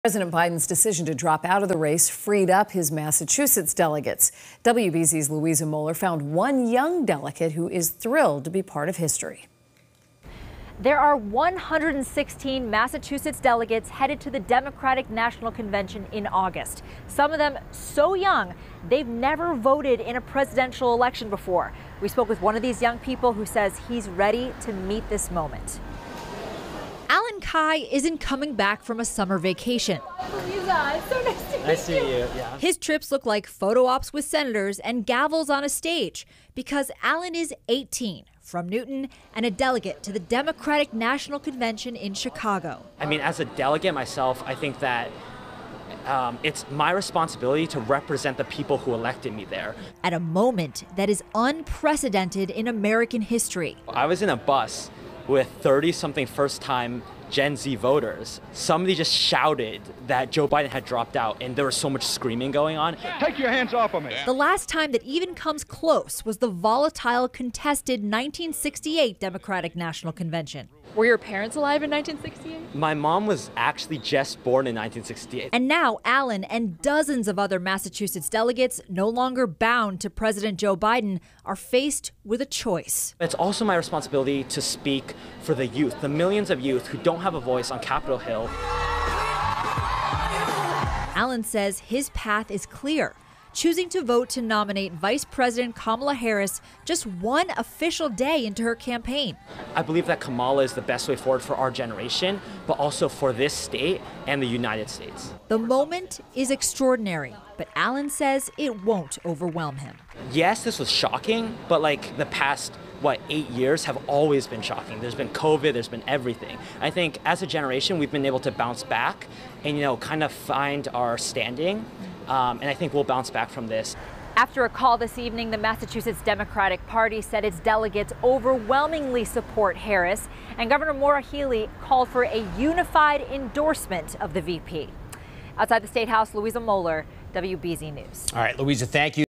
Massachusetts delegates to DNC overwhelmingly support Kamala Harris
Massachusetts delegates to the Democratic National Convention voted overwhelmingly to endorse Vice President Kamala Harris as the 2024 Democratic nominee. WBZ-TV's Louisa Moller reports.